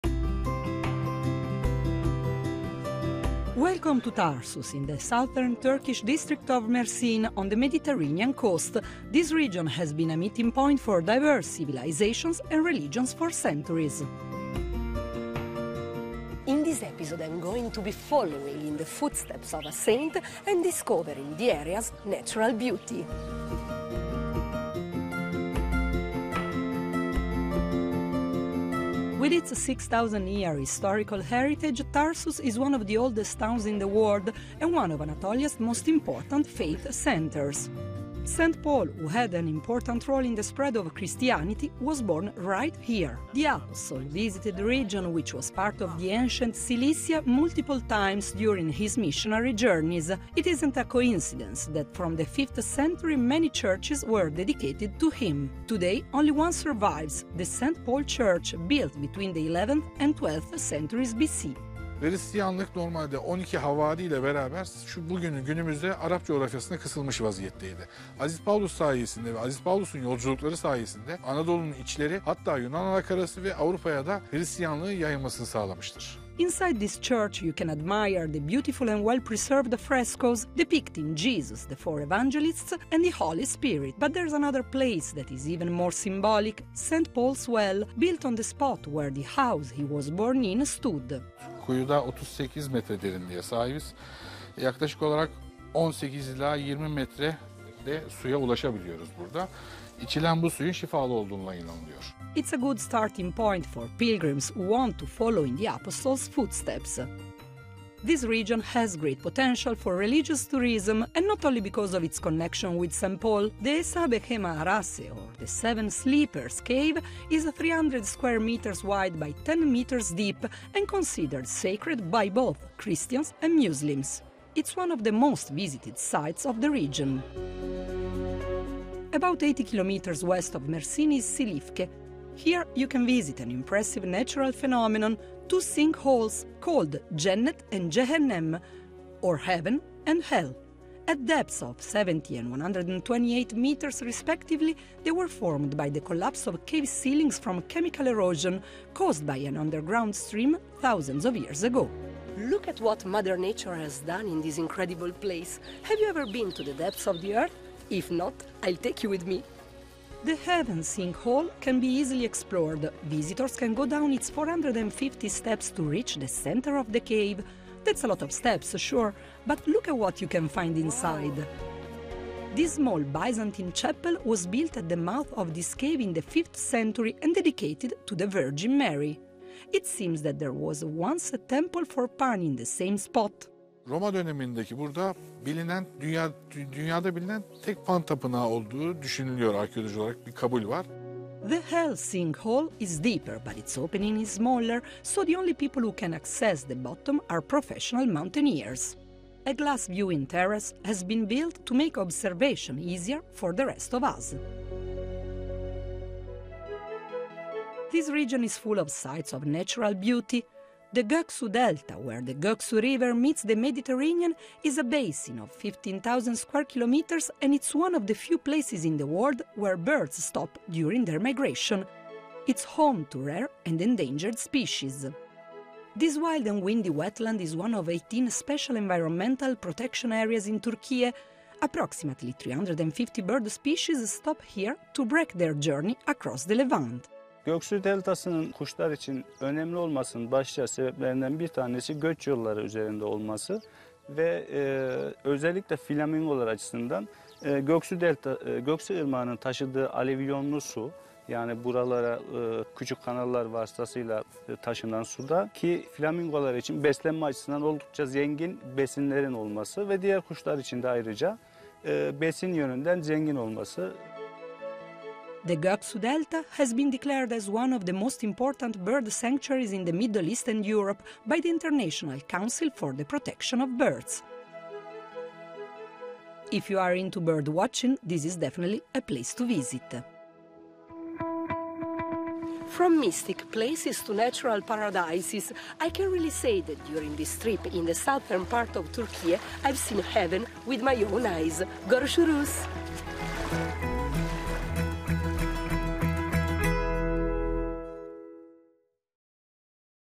به شگفتی‌های طبیعی علاقه دارید؟ پرندگان مهاجر و فروچاله‌های جنوب ترکیه را نباید از دست داد…
منطقه مرسین در ترکیه یک توقفگاه کلیدی برای پرندگان مهاجر است و یکی از مراکز اعتقادی مهم آناتولی است.

GoTürkiyeبا همکاری